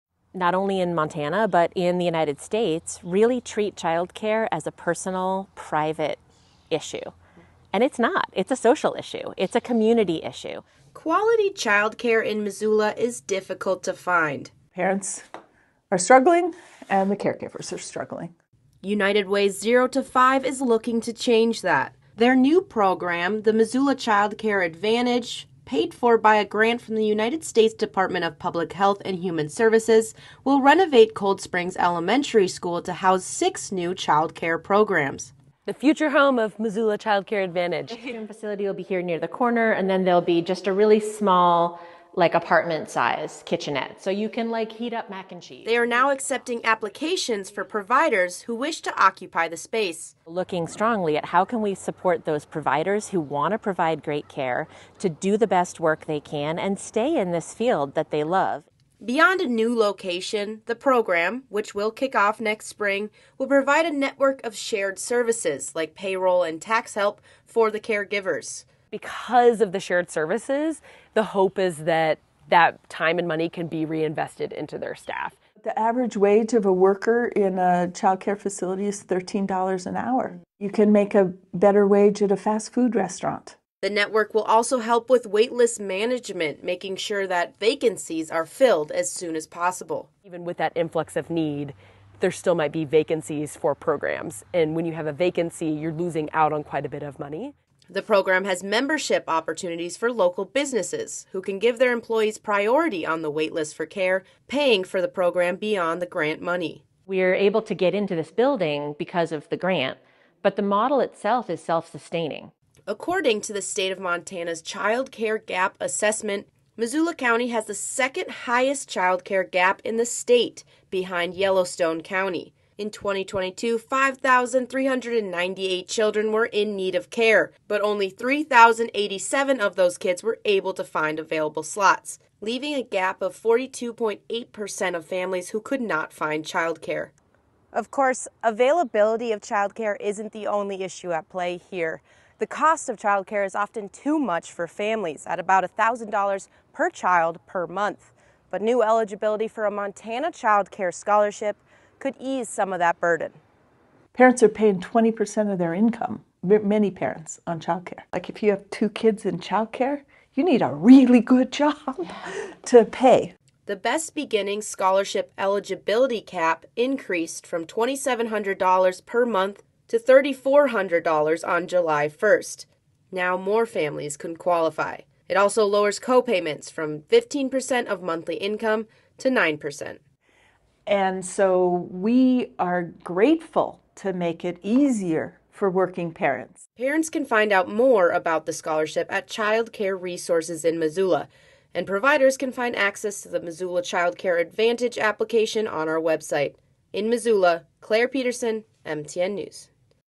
New changes in Missoula look to make child care more accessible
Barriers to receiving high-quality child care is not a new issue, but developing programs in Missoula County and beyond are looking towards long-term solutions.